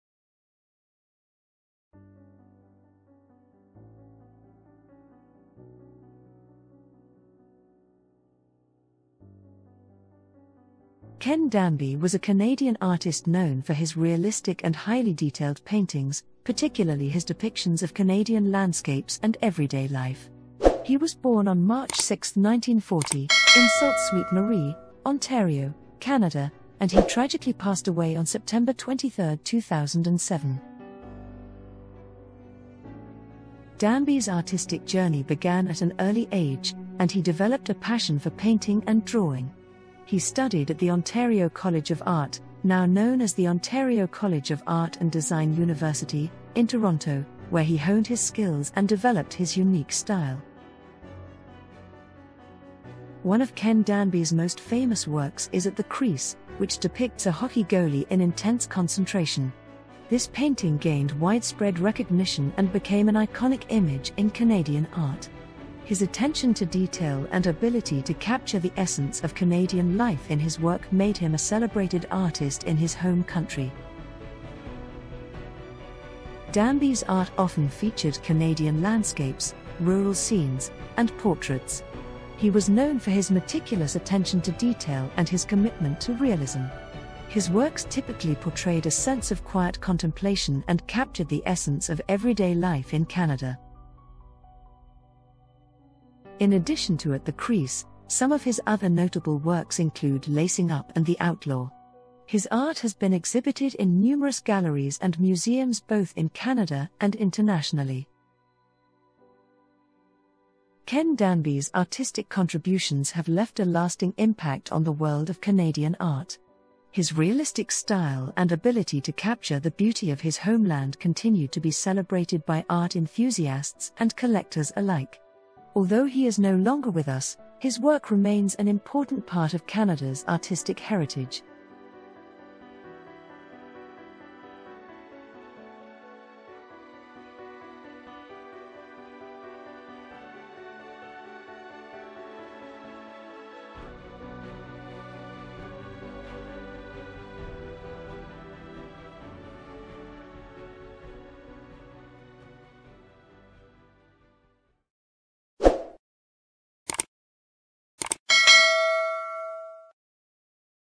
"Ken Danby Realism: The Heartbeat of Canadian Art"
"Ken Danby: Capturing the Essence of Canada in Art"

Join us on a journey through the life and art of Ken Danby, one of Canada's most renowned and beloved artists. Ken Danby, born in Sault Ste. Marie, Ontario, in 1940, left an indelible mark on the Canadian art scene with his remarkable talent for realism and his ability to capture the essence of Canada in his paintings.

In this video, we explore the fascinating story of Ken Danby's artistic journey, from his early years as a budding artist to his rise to fame as a master of realistic art. Discover the iconic paintings that made him famous, including "At the Crease," a timeless representation of the intensity of Canada's favorite sport, hockey.

Through interviews with art experts, glimpses into his studio, and a showcase of some of his most celebrated works, we delve into the techniques and inspirations that shaped Ken Danby's artistry. His meticulous attention to detail and deep connection to Canadian landscapes and culture shine through in each brushstroke.

Whether you're a longtime admirer of Ken Danby's art or new to his work, this video offers a captivating exploration of his life and legacy. Join us in celebrating the artistic genius of Ken Danby and his enduring contribution to the world of Canadian art.